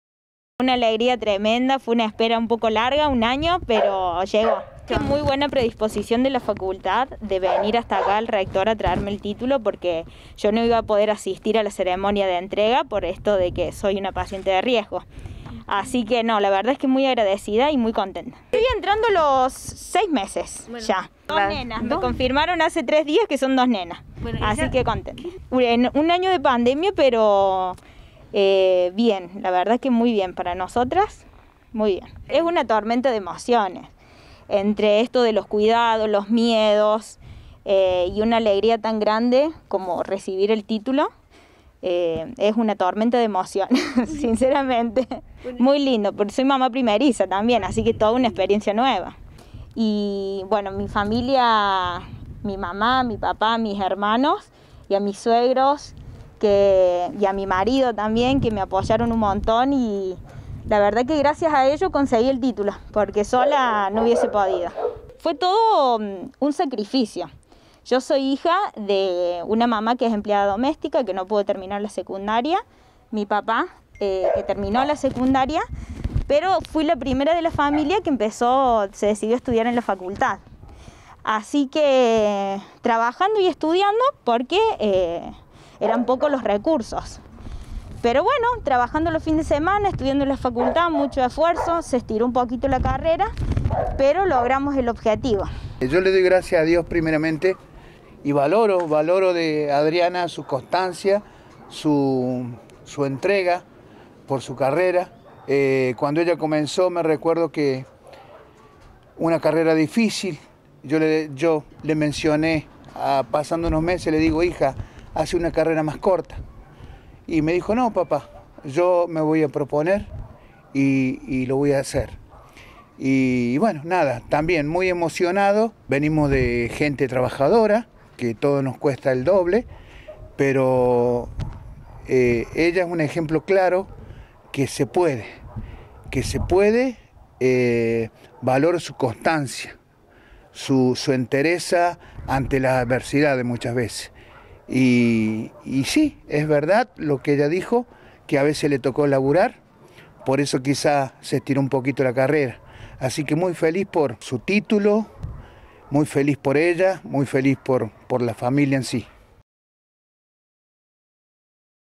EGRESAR EN TIEMPOS DE PANDEMIA
La UNSL con sede en Villa Mercedes, realizo este miércoles una entrega especial de diplomas a sus egresados. Un caso muy particular es el de Adriana Jofre, quien está embarazada de mellizas y es grupo de riesgo, debido a esto recibió su diploma de Ingeniera Industrial en su hogar junto a la familia.